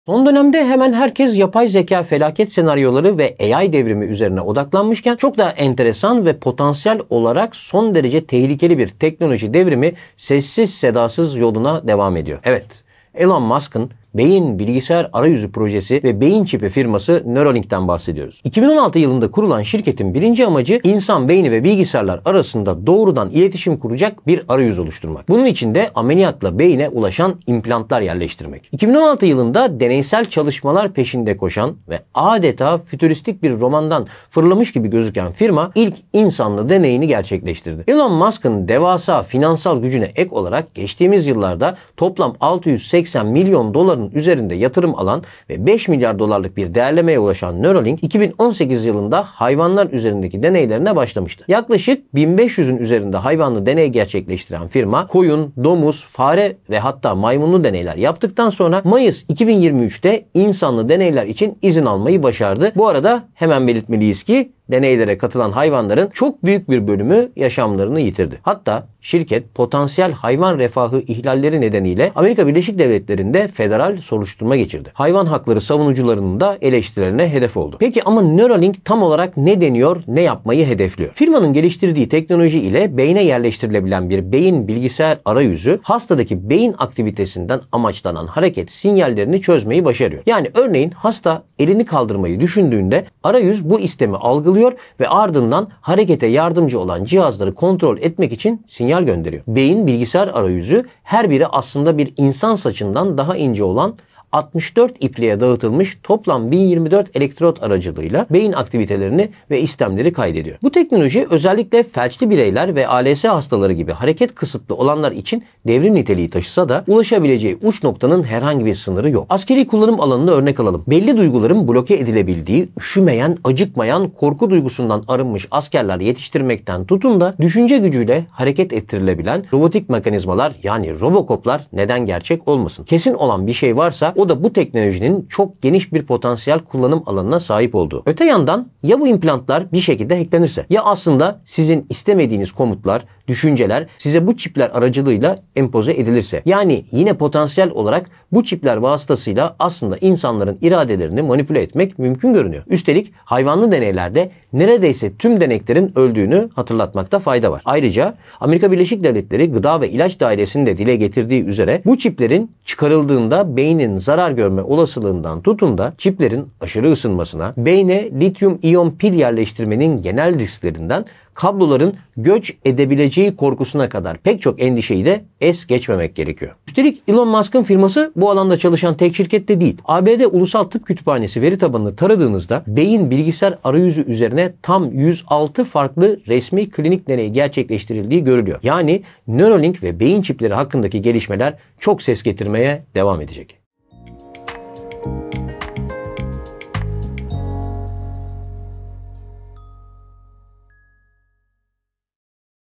Sonunda oldu! Neuralink insan beynine çip taktı. Peki şimdi ne olacak?
Son dönemde hemen herkes yapay zekâ felaket senaryoları ve AI devrimi üzerine odaklanmışken, çok daha enteresan ve potansiyel olarak son derece tehlikeli bir teknoloji devrimi sessiz sedasız yoluna devam ediyor.

Bunun için de ameliyatla beyine ulaşan implantlar yerleştirmek. 2016 yılında deneysel çalışmalar peşinde koşan ve adeta futuristik bir romandan fırlamış gibi gözüken Neuralink ilk insanlı deneyini gerçekleştirdi.

Elon Musk’ın devasa finansal gücüne ek olarak geçtiğimiz yıllarda toplam 680 milyon doların üzerinde yatırım alan ve 5 milyar dolarlık bir değerlemeye ulaşan Neuralink, 2018 yılında hayvanlar üzerindeki deneylerine başlamıştı. Yaklaşık 1.500’ün üzerinde hayvanlı deney gerçekleştiren firma koyun, domuz, fare ve hatta maymunlu deneyler yaptıktan sonra Mayıs 2023’te insanlı deneyler için izin almayı başardı ve ilk testini gerçekleştirdi.  Bu arada, hemen belirtmeliyiz ki deneylere katılan hayvanların çok büyük bir bölümü yaşamlarını yitirdi. Hatta şirket, potansiyel hayvan refahı ihlalleri nedeniyle ABD’de federal soruşturma geçirdi. Hayvan hakları savunucularının da eleştirilerine hedef oldu.

Peki ama Neuralink tam olarak ne deniyor, ne yapmayı hedefliyor?

Frimanın geliştirdiği teknoloji ile beyne yerleştirilebilen bir beyin bilgisayar arayüzü (BCI), hastadaki beyin aktivitesinden amaçlanan hareket sinyallerini çözmeyi başarıyor. Yani örneğin hasta elini kaldırmayı düşündüğünde arayüz bu istemi algılıyor ve ardından harekete yardımcı olan cihazları kontrol etmek için sinyal gönderiyor. Beyin bilgisayar arayüzü, her biri aslında bir insan saçından daha ince olan 64 ipliğe dağıtılmış toplam 1024 elektrot aracılığıyla beyin aktivitelerini ve istemleri kaydediyor.

Bu teknoloji özellikle felçli bireyler ve ALS hastaları gibi hareket kısıtı olanlar için devrim niteliği taşısa da, ulaşabileceği uç noktanın herhangi bir sınırı yok. Askeri kullanım alanını örnek alalım. Belli duyguların bloke edilebildiği üşümeyen, acıkmayan, korku duygusundan arınmış askerler yetiştirmekten tutun da düşünce gücüyle hareket ettirilebilen robotik mekanizmalara yani Robocop’lar neden gerçek olmasın? Kesin olan bir şey varsa o da bu teknolojinin çok geniş bir potansiyel kullanım alanına sahip olduğu.

Öte yandan, ya bu implantlar bir şekilde hacklenirse? Ya aslında sizin istemediğiniz komutlar (düşünceler) size bu çipler aracılığıyla empoze edilirse? Yani, yine potansiyel olarak bu çipler vasıtasıyla aslında insanların iradelerini manipüle etmek mümkün gözüküyor. Üstelik hayvanlı deneylerde neredeyse tüm deneklerin öldüğünü hatırlatmakta fayda var. Ayrıca ABD Gıda ve İlaç Dairesi’nin de dile getirdiği üzere, bu çiplerin çıkarıldığında beynin zarar görmesi olaslığından tutun da çiplerin aşırı ısınmasına, beyne lityum iyon pil yerleştirmenin genel risklerinden kabloların “göç edebileceği” korkusuna kadar pek çok endişeyi de es geçmemek gerekiyor.

Üstelik Elon Musk’ın firması bu alanda çalışan tek şirket de değil. ABD Ulusal Tıp Kütüphanesi veri tabanını taradığınızda beyin bilgisayar arayüzü üzerine tam 106 farklı resmi klinik deney gerçekleştirildiği görülüyor. Yani Neuralink ve beyin çipleri hakkındaki gelişmeler çok ses getirmeye devam edecek.